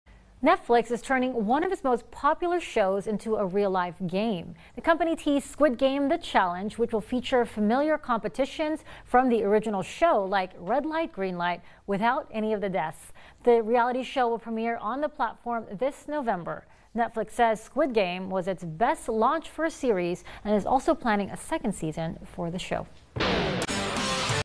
SQUID GAME REALITY SHOW
NETFLIX IS TURNING ONE OF ITS MOST POPULAR SHOWS INTO A REAL-LIFE GAME.